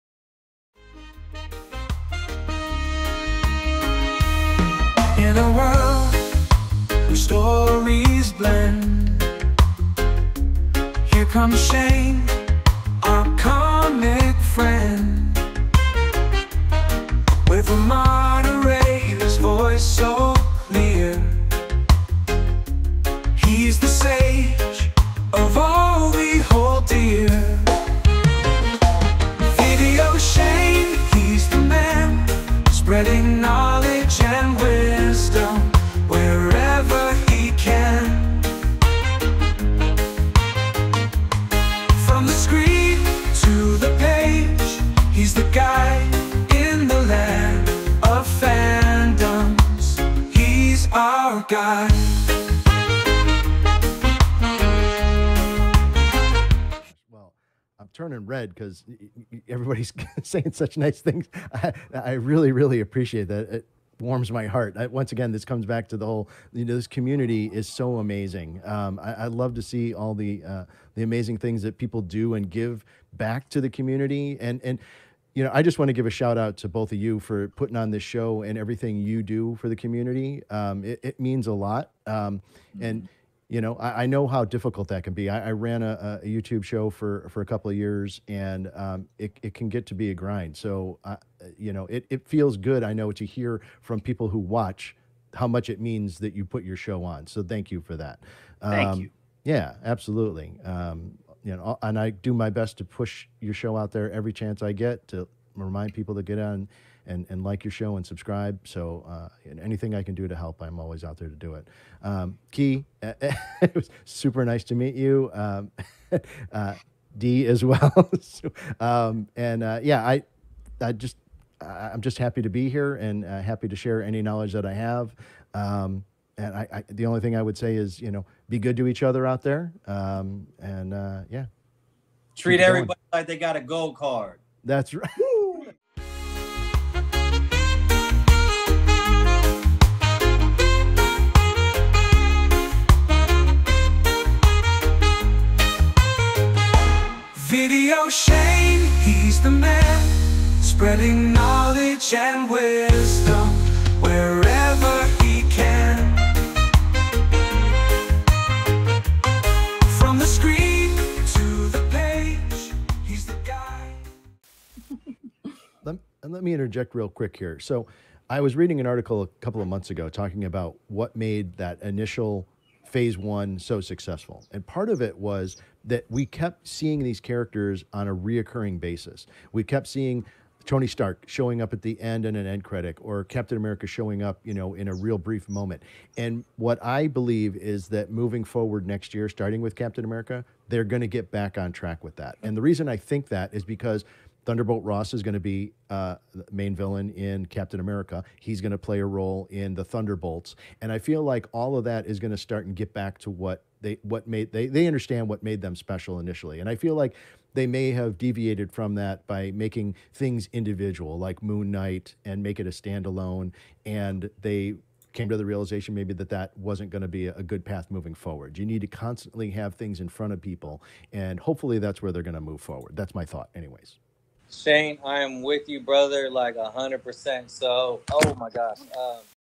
Community Center Stage. VideoShane.
Today's Community Center Stage is focused on VideoShane.

Digital collector, tv & movie enthusiast. Moderator with wisdom and kindness.  Check out his YouTube and X Socials for coverage on MacFarlane Toys Digital, Discord trivia nights and all things fandom.